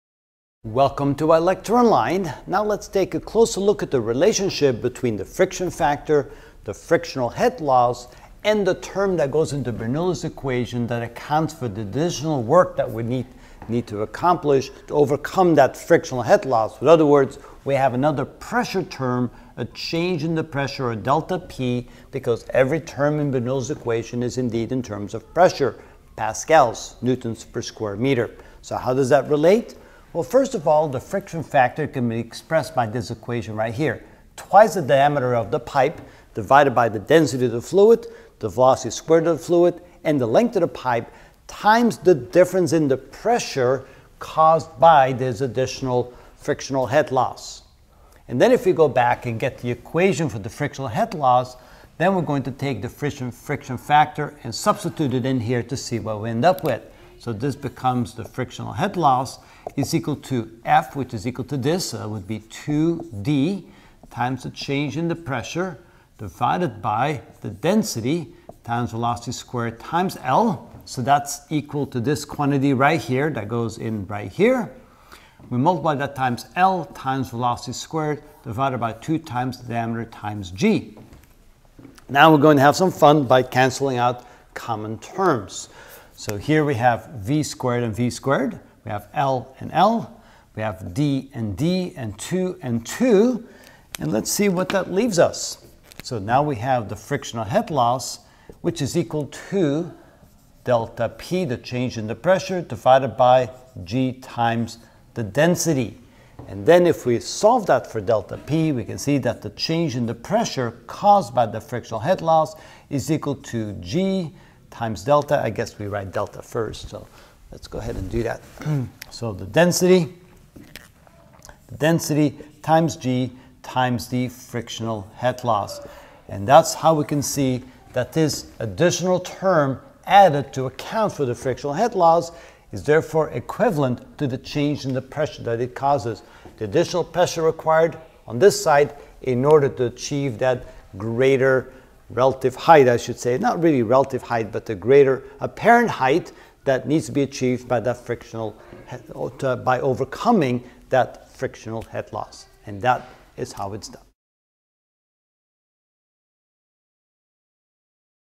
Physics 34.1 Bernoulli's Equation & Flow in Pipes (9 of 38) Friction Factor Equation
In this video I will explain the friction factor, frictional head loss, and the term in Bernoulli's equation that accounts for the word need to overcome the frictional head loss.